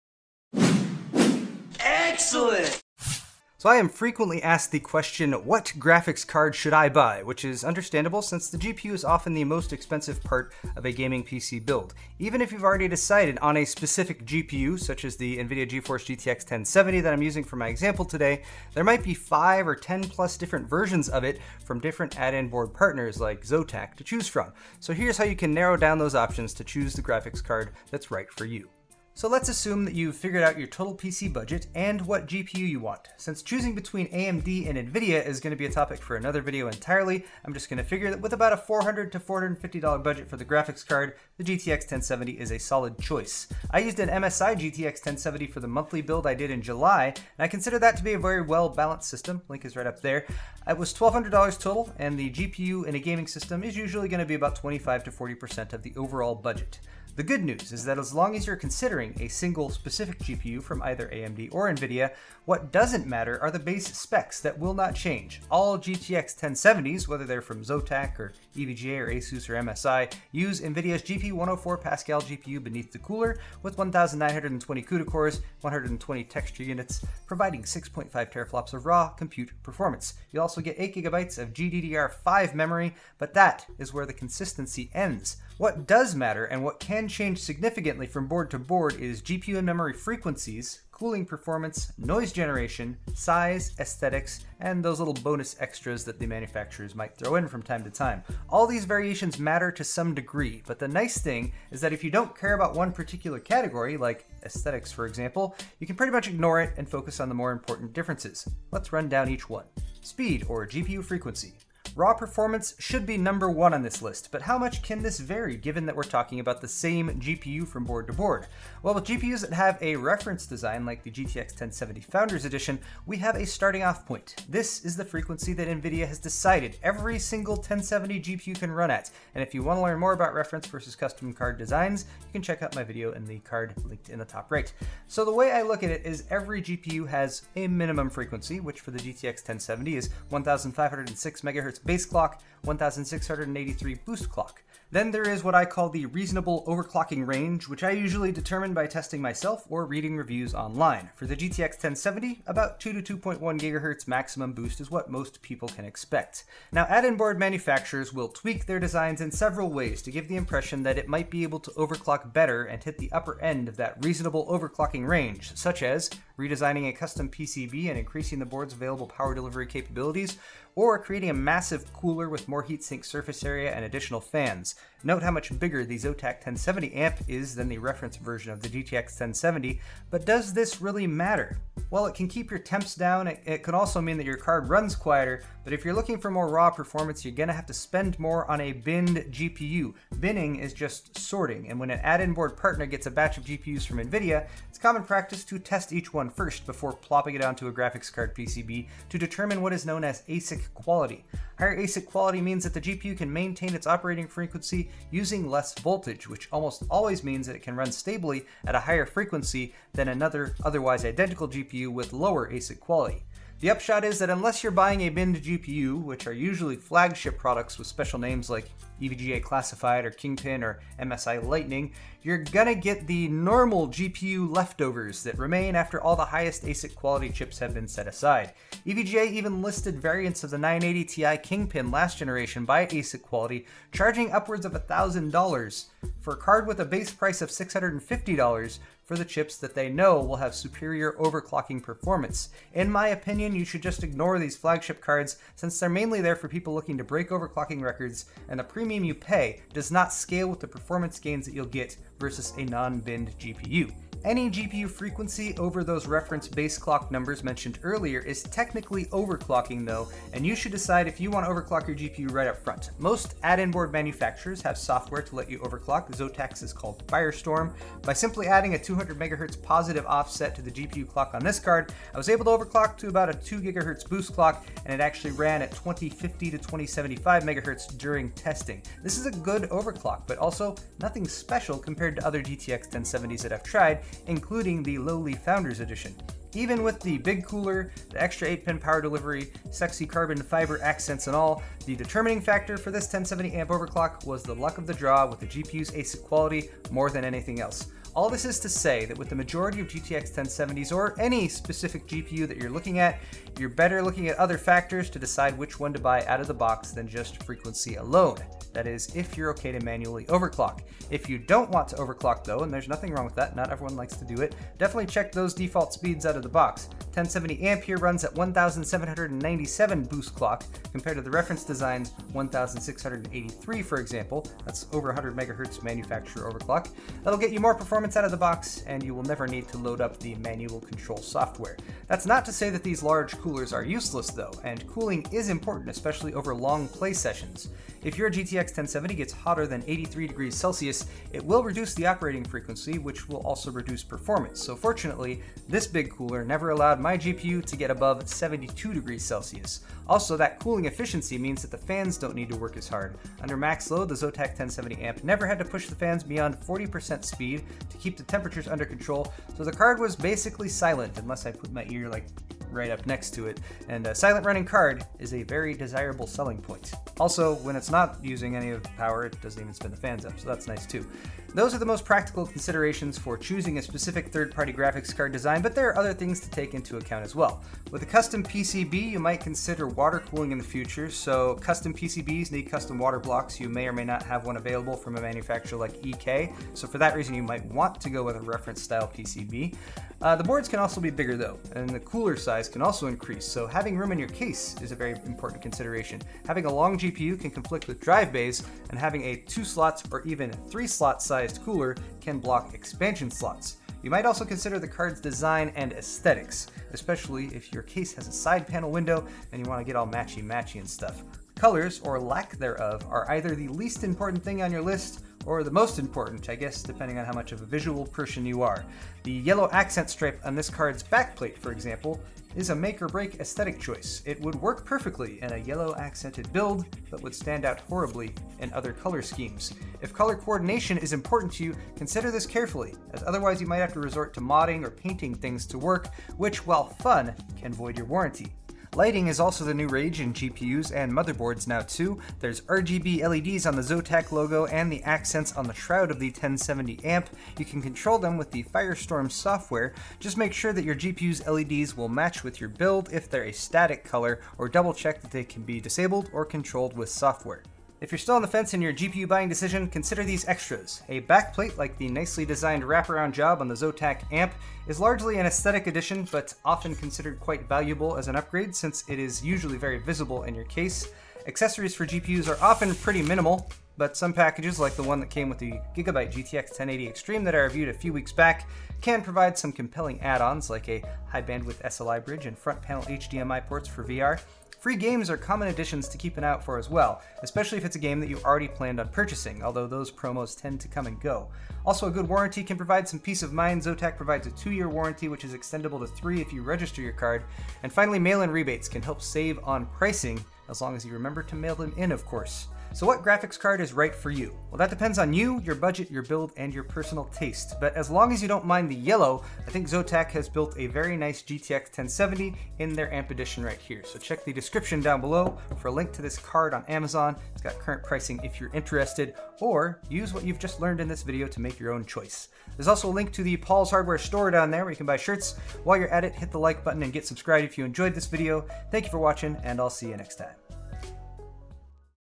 How To Choose The Best Graphics Card (For You!)
ft. the Zotac GTX 1070 AMP!

Here's a guide to walk you through the key points to consider when narrowing down your graphics card choice -- because after you've chosen your GPU, you'll still probably have 10 to 15 graphics card variants to choose from. Enjoy!

Zotac GTX 1070 AMP! @ Amazon